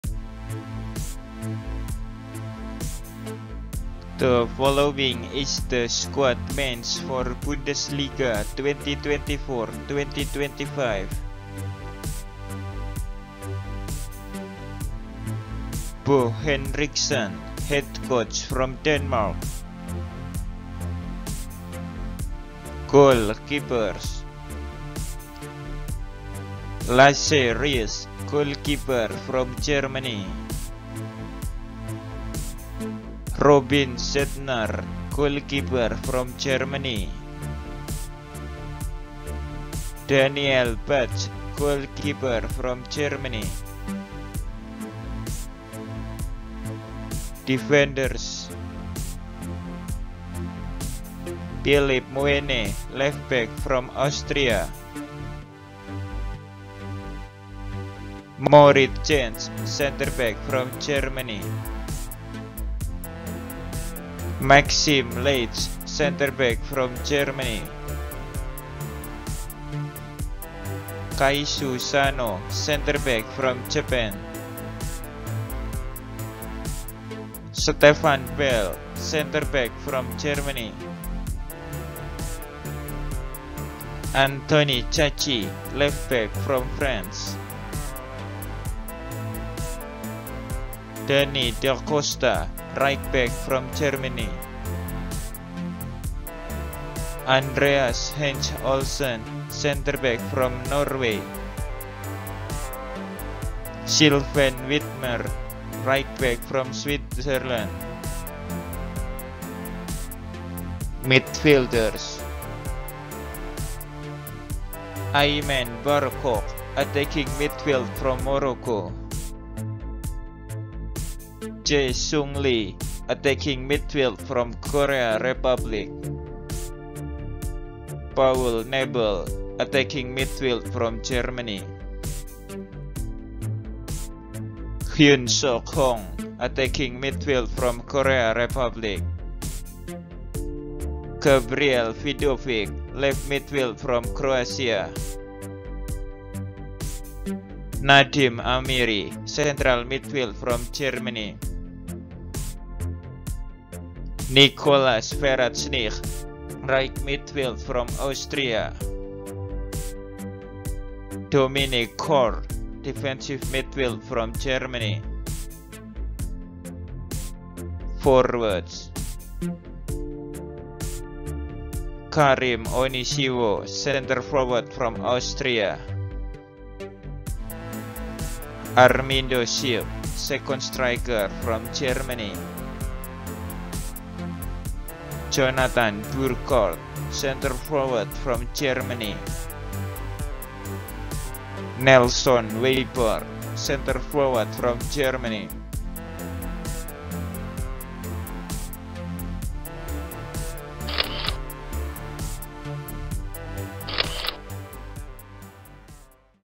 FSV MAINZ 05 🔴⚪️ MEN SQUAD TEAMS - Bundesliga Season 2024/2025 with Update Transfer | FAN Football
Hello Guys, This video is Squad FSV Mainz 05 for Bundesliga Season 2024/2025

Head Coach :
Bo Henriksen (Denmark)

Goalkeepers :
Lasse Rieß (Germany)
Robin Zentner (Germany)
Daniel Batz (Germany)

Defenders :
Philipp Mwene (Austria)
Moritz Jenz (Germany)
Maxim Leitsch (Germany)
Kaishu Sano (Japan)
Stefan Bell (Germany)
Anthony Caci (France)
Danny Da Costa (Germany)
Andreas Hanche-Olsen (Norway)
Silvan Widmer (Austria)

Midfielders :
Aymen Barkok (Morocco)
Jae-Sung Lee (South Korea)
Paul Nebel (Germany)
Hyun-Seok Hong (South Korea)
Gabriel Vidovic (Croatia)
Nadiem Amiri (Germany)
Nikolas Veratschnig (Austria)
Dominik Kohr (Germany)

Forwards :
Karim Onisiwo (Austria)
Armindo Sieb (Germany)
Jonathan Burkardt (Germany)
Nelson Weiper (Germany)